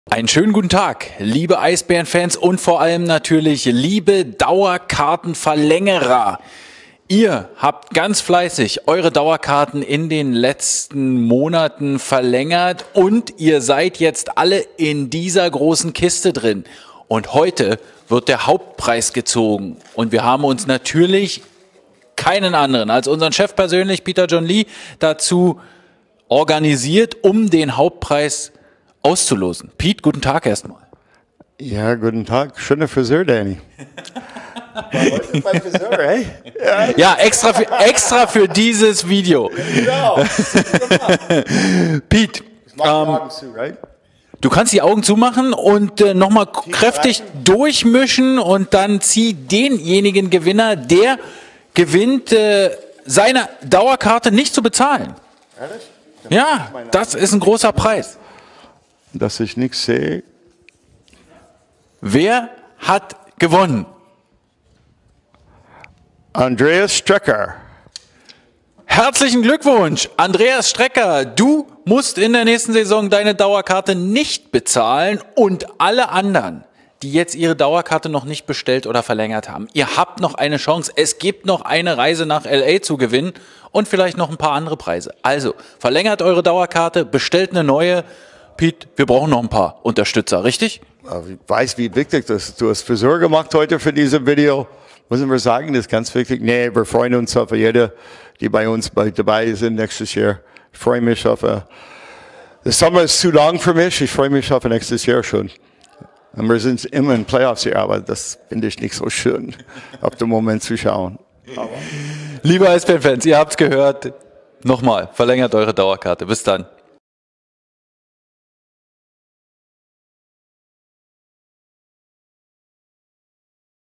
20 (fast) unbezahlbare Preise - Dauerkarte kostenfrei
Die allerletzte Verlosung unserer 20 (fast) unbezahlbaren Preise für Dauerkartenverlängerer für die Saison 2019/20 nahm Eisbären-Chef Peter John Lee persönlich vor. Der Hauptpreis, ein Jahr die Dauerkarte kostenfrei, geht an...

Neue Dauerkarten können noch 10 Tage lange (also bis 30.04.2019) zum Frühbucherpreis bestellt werden!